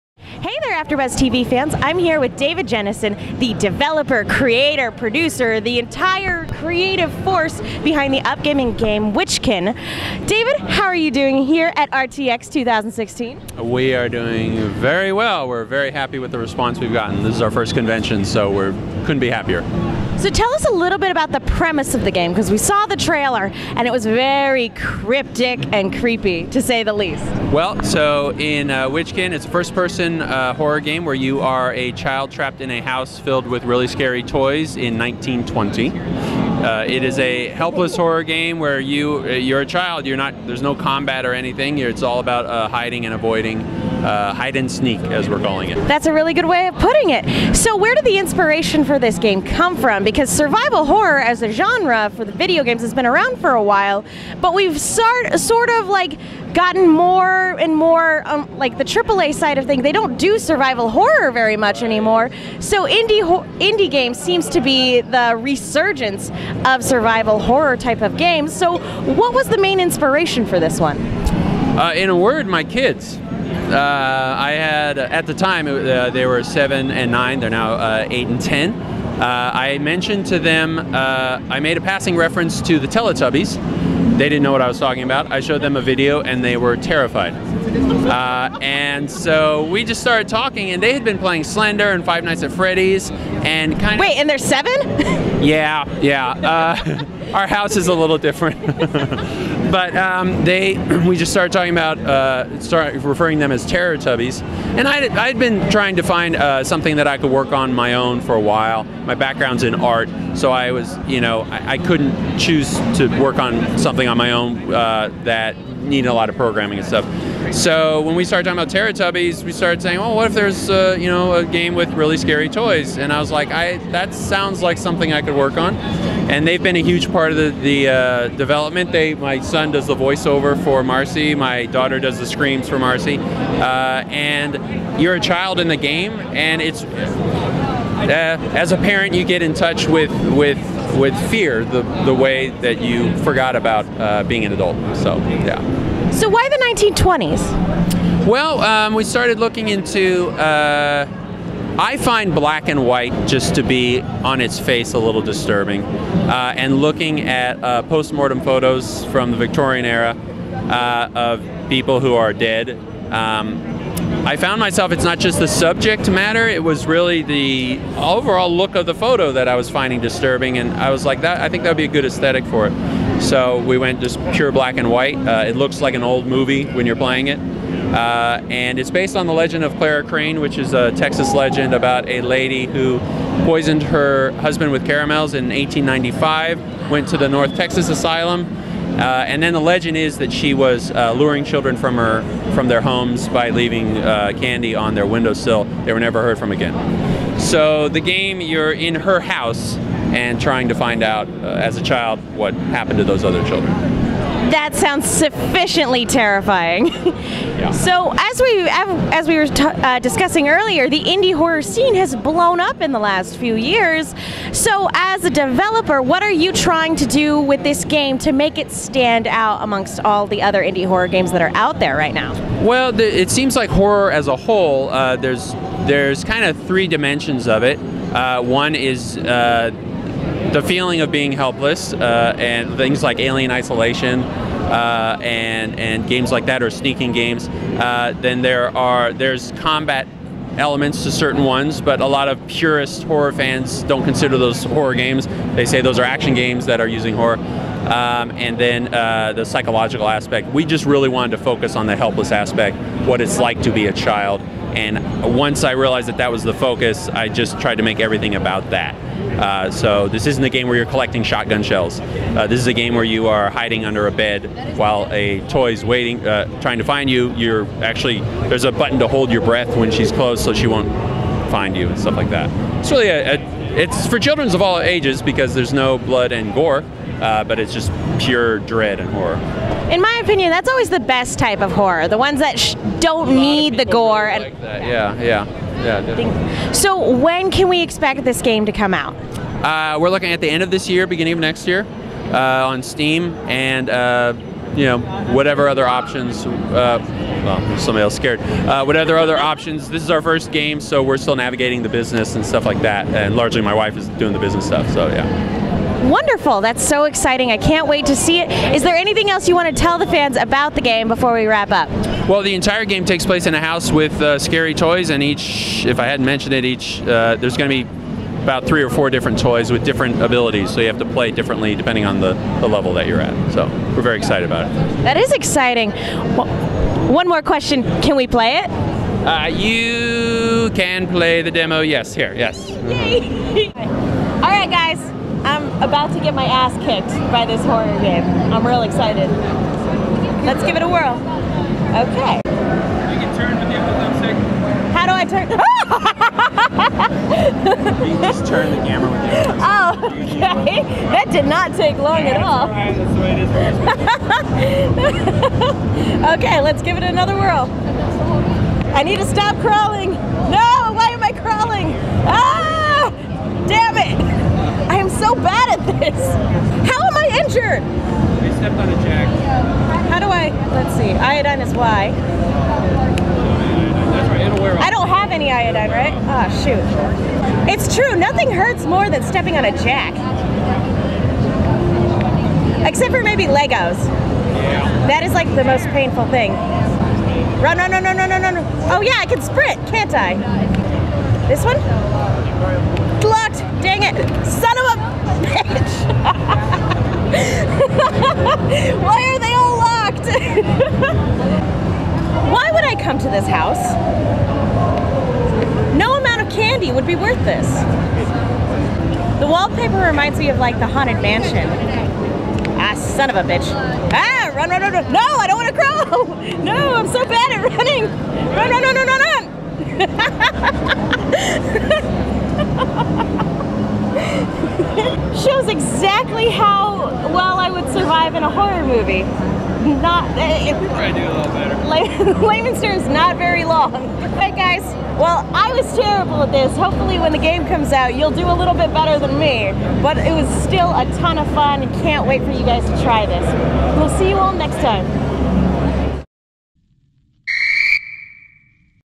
David Jennison and The Witchkin @ RTX 2016
Afterbuzz TV's Megan Salinas sits down to talk with David Jennison of Coven Games about his upcoming game The Witchkin. Can Megan survive the terrifying demo of this game? Tune in to find out!